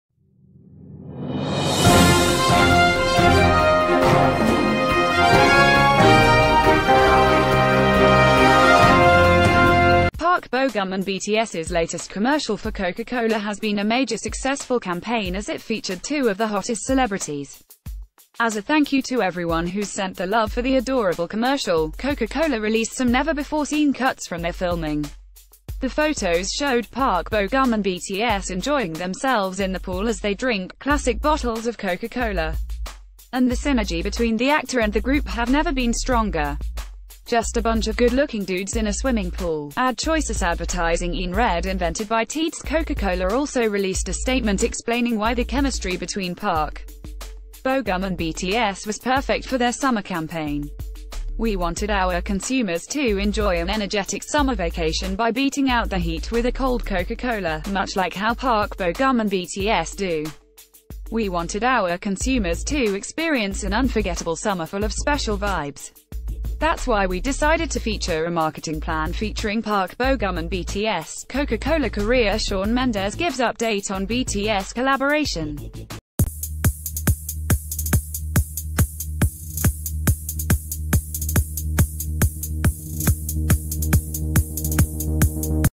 Coca-Cola Releases Special Behind Cuts Of Park Bo Gum And BTS
Park Bo Gum and BTS‘s latest commercial for Coca-Cola has been a major successful campaign as it fea...